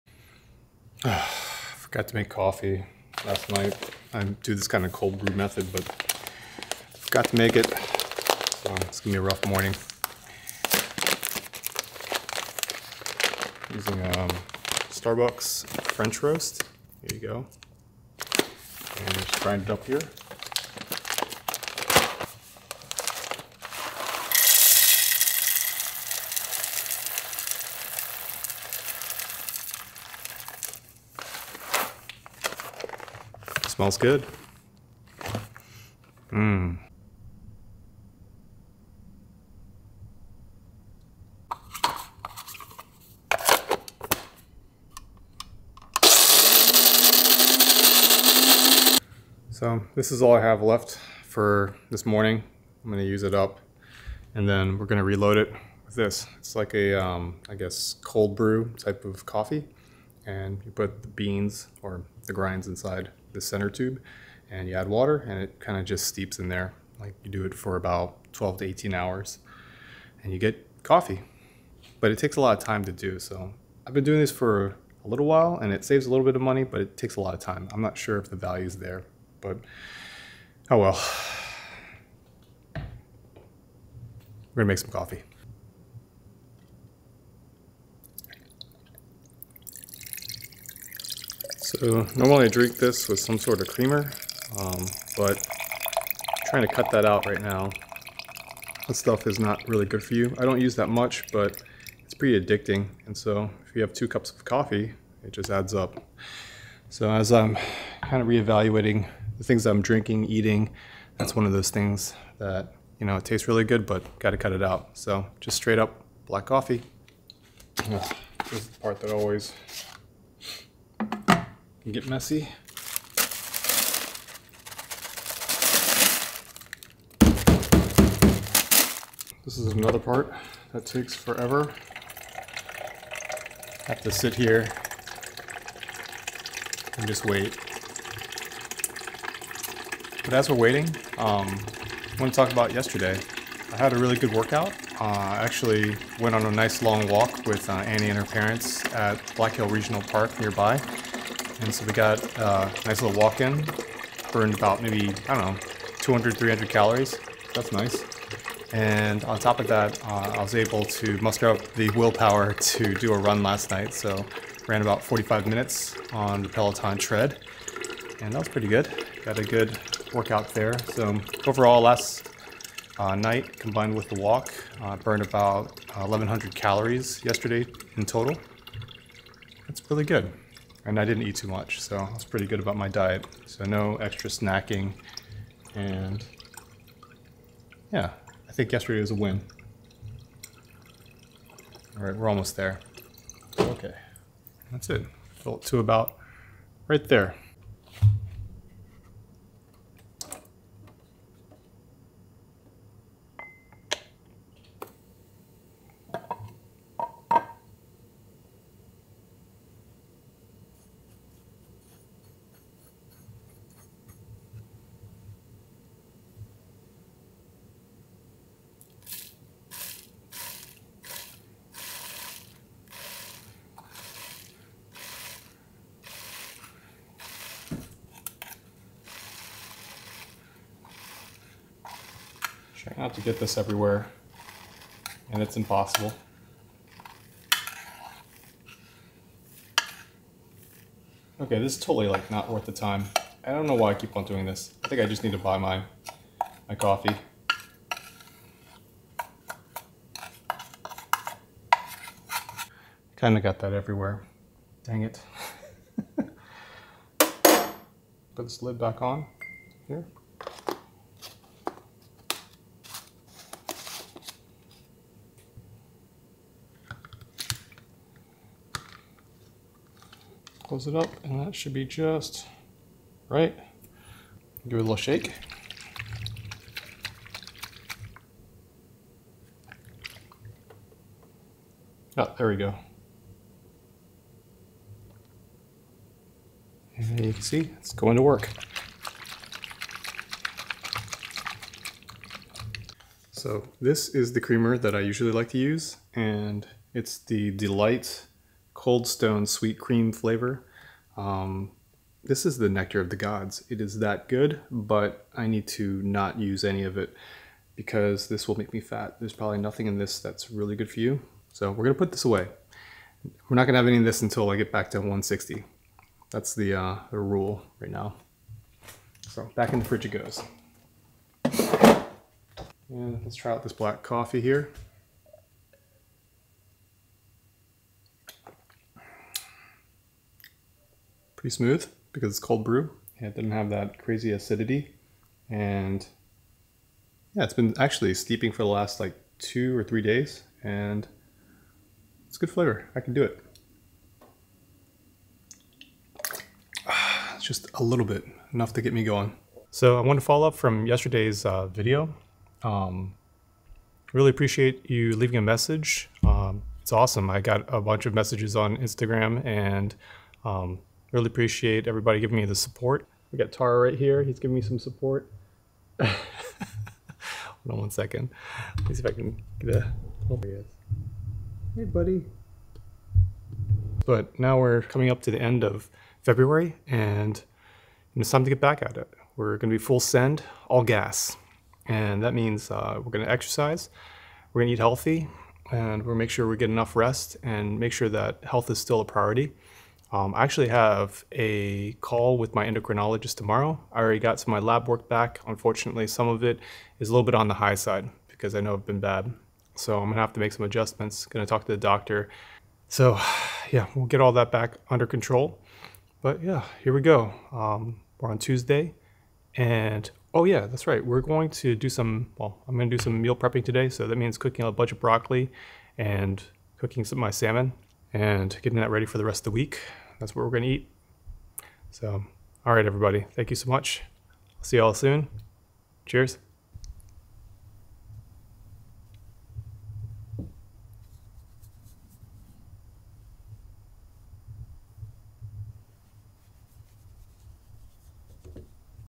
making cold brew coffee at home is inefficient
I forgot to make my cold brew coffee last night. I'm not sure why I do this to myself. I think I need to go back to buying bottles of cold brew coffee from the store. The process of grinding my own beans and making my own cold brew just eats up too much time.

But besides that, it's a good day—I burnt over 1100 calories yesterday, and my diet is back on track. Let's keep it up fam.